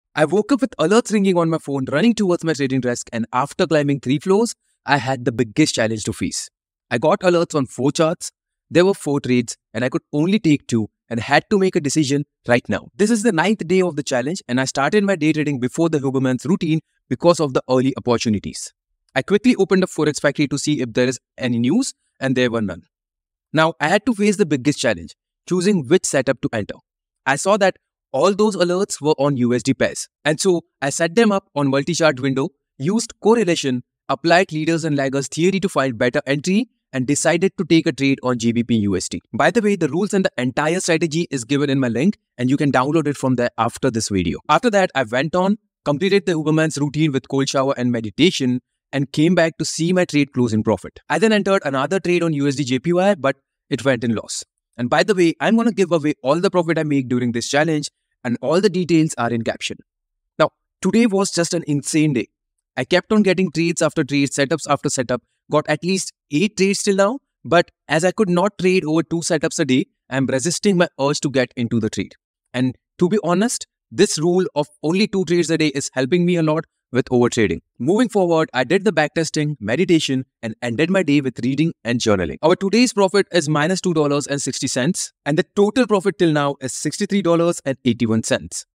9/30 Trading Challenge for Success
Daksh Trader's 30-Day Trading Challenge | DAY 9 📈🔥
Hey everyone! Welcome back to Day 9 of my 30-Day Trading Challenge. Today's been a roller coaster of emotions and market movements, but that's what makes this challenge so thrilling! Dive in to see the ups, downs, strategies, and insights as I navigate today's trading landscape.

Daily insights into my trading strategies and decision-making processes
Real-time reactions to market changes
Honest reflections on wins, losses, and lessons learned